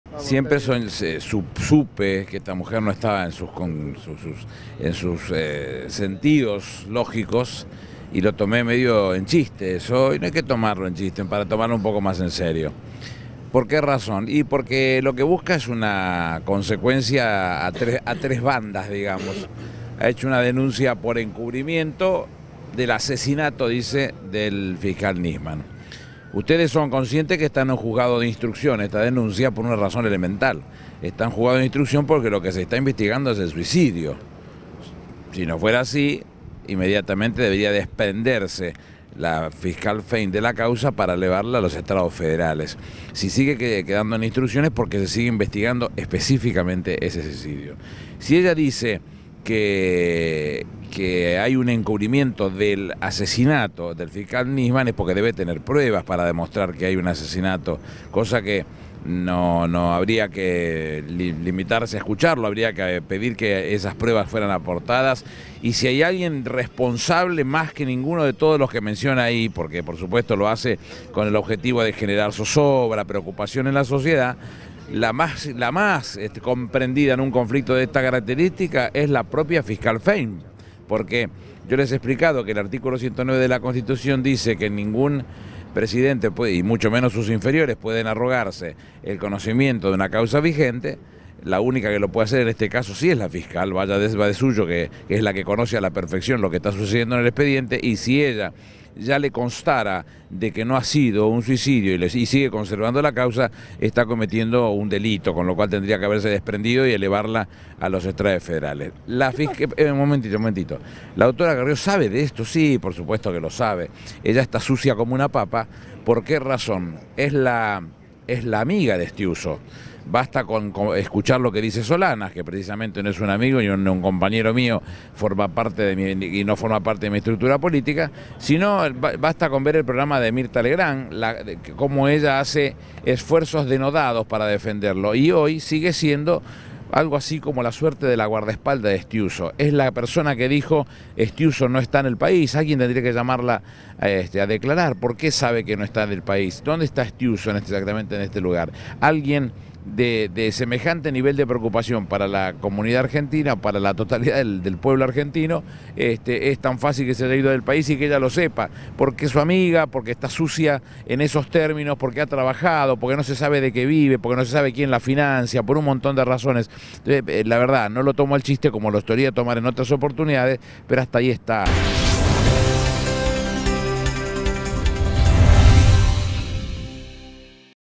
Aníbal Fernández: “Carrió está sucia como una papa”
El secretario general de la Presidencia, Aníbal Fernández, afirmó hoy que la diputada nacional por la Coalición Cívica Elisa Carrió "está más sucia que una papa" por obrar "como una guardaespaldas de Jaime Stiuso" y "hace esfuerzos denodados por defenderlo".

Fernández descalificó la denuncia que la legisladora presentará hoy contra la presidenta Cristina Fernández de Kirchner y algunos de sus funcionarios por presunto encubrimiento en la muerte del fiscal federal Alberto Nisman, al señalar que "lo hace para generar zozobra".

"Carrió es amiga de Stiuso. Basta con escuchar lo que dijo (Fernando) Solanas, que precisamente no es un amigo ni un compañero que forme parte de mi estructura política. Basta también con ver el programa de Mirtha Legrand y escuchar cómo ella hace esfuerzos denodados para defenderlo, y hoy sigue siendo algo así como la guardaespaldas de Stiuso", dijo el funcionario en declaraciones a los periodistas acreditados en Casa de Gobierno.

Asimismo, agregó que Carrió "es la persona que dijo que Stiuso no estaba en el país", por lo que indicó: "Alguien tendría que llamarla a declarar. ¿Cómo sabe que Stiuso no está en el país? ¿Dónde está exactamente ahora? Ella lo sabe, porque es su amiga, porque está sucia en esos términos, porque no se sabe de qué vive, quién la financia y por un montón de razones".

Por otra parte, el secretario general de la Presidenta reiteró que "inmediatamente" que el Gobierno se enteró que Stiuso había sido citado a declarar por la fiscal del caso Nisman, Viviana Fein, el titular de la Secretaría de Inteligencia (SI), Oscar Parrilli, se "presentó y lo liberó para que pueda expresarse", al tiempo que ponderó que "el Gobierno ha sido generoso en ese sentido" porque ahora el ex espía "puede hablar del tema que le pregunten sobre sus 42 años de servicio".

Por otra parte, consultado por la marcha convocada por los fiscales por la muerte de Nisman, Fernández dijo que "no hay nada malo con eso" y que "forma parte de los derechos que hemos conquistado todos los argentinos", aunque aprovechó la pregunta para criticar que "entre ellos hay gente que no se movió por otros argentinos en los momentos más duros de la democracia".

"Gente como (Julio) Strassera, que ha rechazado hábeas corpus. Recuerdo el caso de Jorge Cepernic -ex gobernador de Santa Cruz- que me lo ha comentado personalmente: había venido a Buenos Aires y había ido al cine, cuando fue detenido por las actas de reparación institucional, algo sin sentido que no apuntaba ningún delito en especial. Strassera consideró que eso era insuficiente y rechazó el hábeas corpus", dijo el funcionario nacional y ex senador.

Por último, Fernández agregó en forma irónica sobre la figura del fiscal del juicio a las juntas militares, que "ahora es él el que va a caminar y marchar; bienvenido sea que se haya dado cuenta que la libertad y el respeto por las instituciones sean otra cosa".